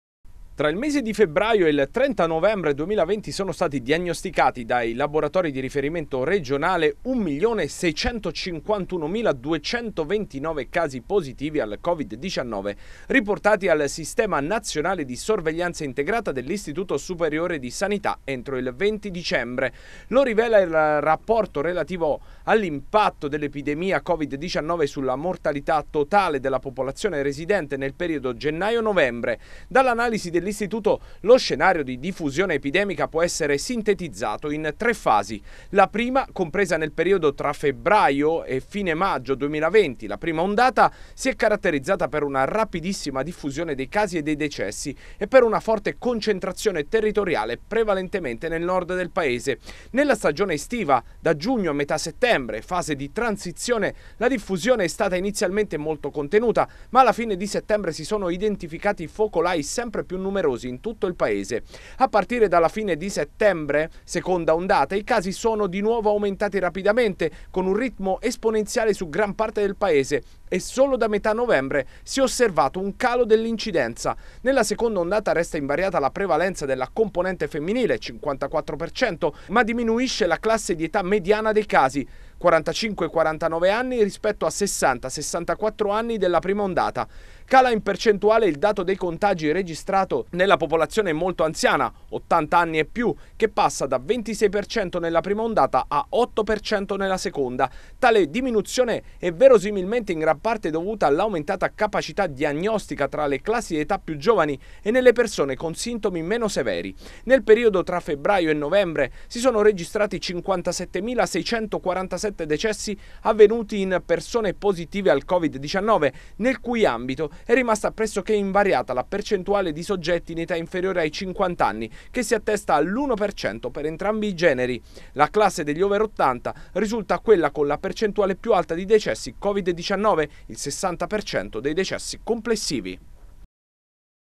Istat-Iss: tra febbraio e novembre oltre 1,5 milioni di positivi
In 10 mesi di emergenza sanitaria, oltre un milione e mezzo di contagi in Italia. Questo emerge dal rapporto relativo all’impatto dell’epidemia eleborato da Istat e Istituto Superiore di Sanità. Ripercorriamo le tre fasi della pandemia con le relative caratterizzazioni